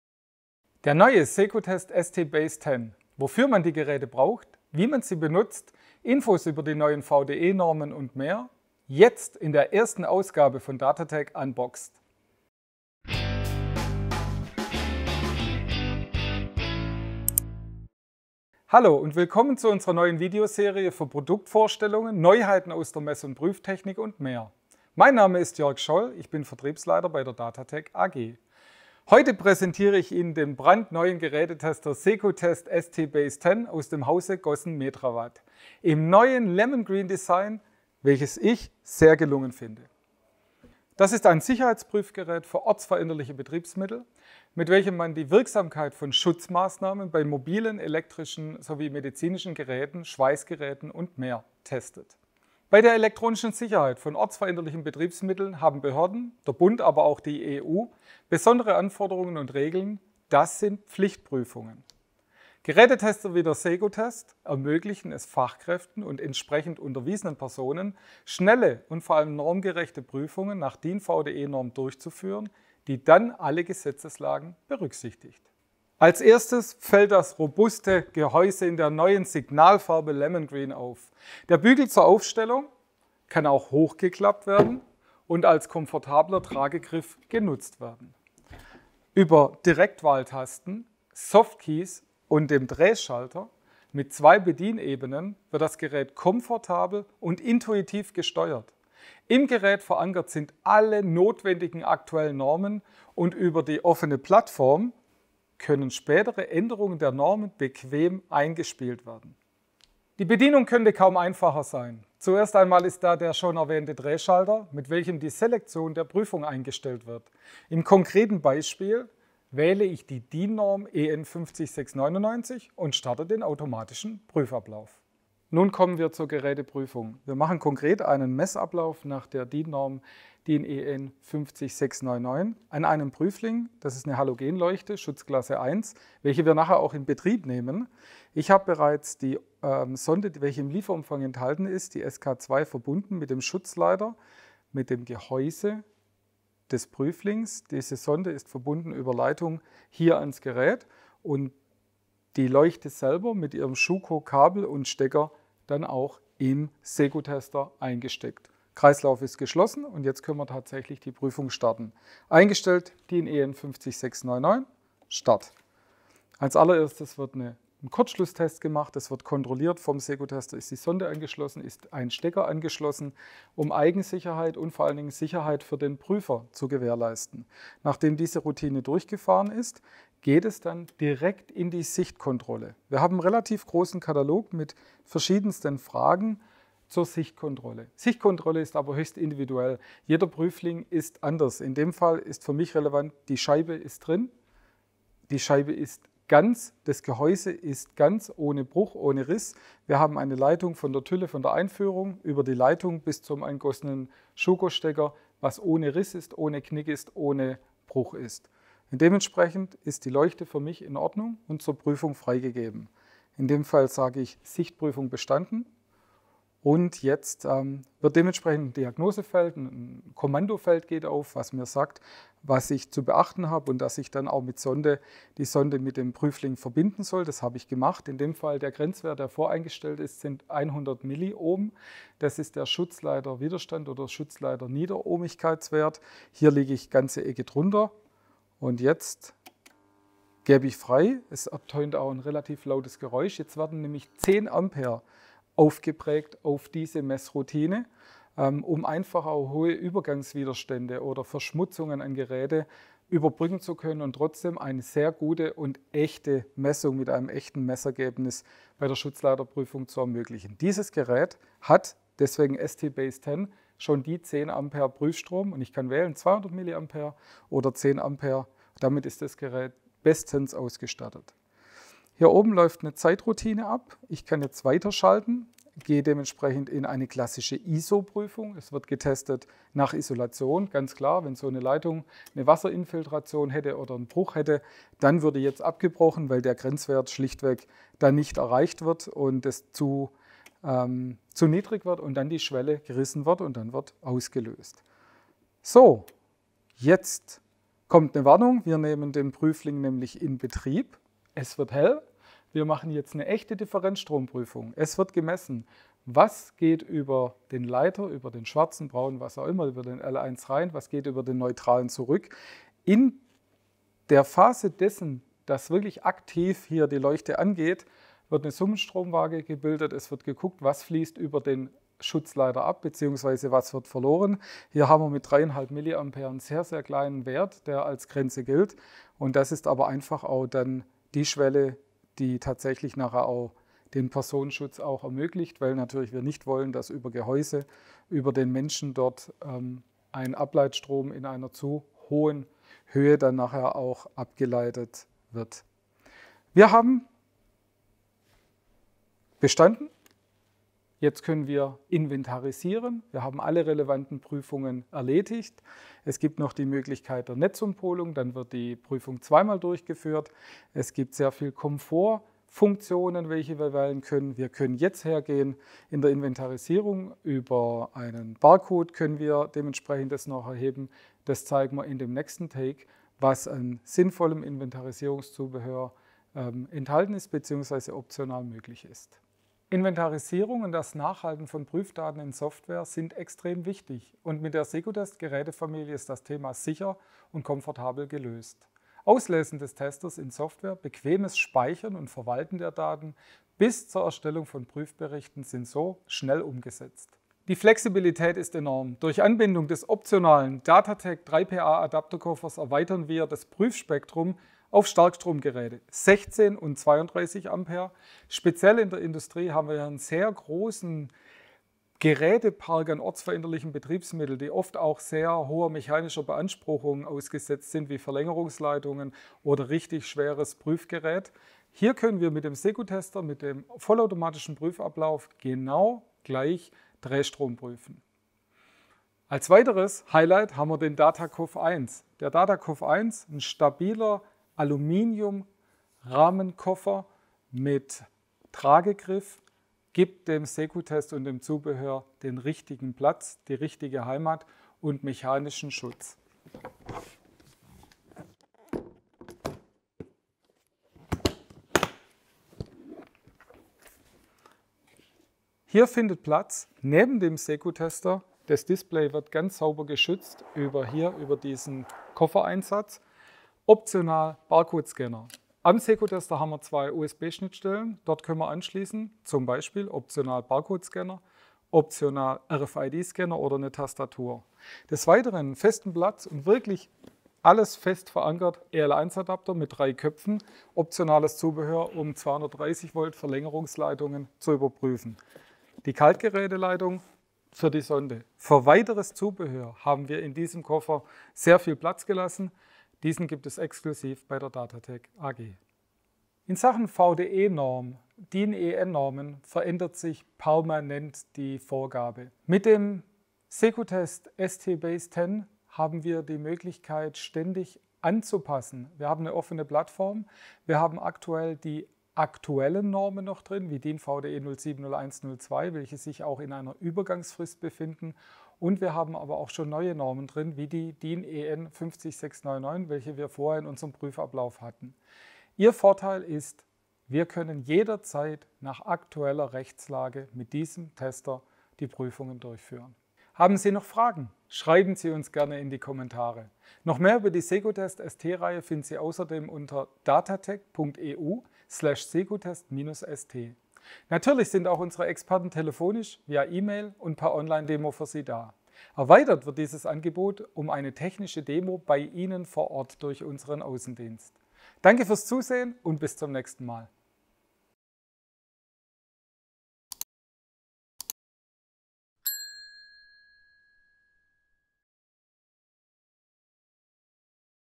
dataTec unboxed - Exklusive Weltpremiere des neuen Secutest ST Base 10 von Gossen Metrawatt.
dataTec unboxed ist die neue Video-Serie von dataTec für Produtkvorstellungen und Neuigkeiten aus der Mess- und Prüftechnik. In diesem ersten Video stellen wir den brandneuen Secutest ST Base 10 von Gossen Metrawatt vor.

Sie wollen persönliche Beratung am Telefon, am Bildschirm, oder vor Ort?  Kontaktieren Sie uns

0:00 Intro/Begrüßung
0:28 Vorstellung Secutest ST Base 10
1:15 Design & Bedienung
2:04 Praxisbeispiel
6:19 Software
6:36 Exklusives Zubehör
8:15 VDE-Normen
8:53 Noch Fragen?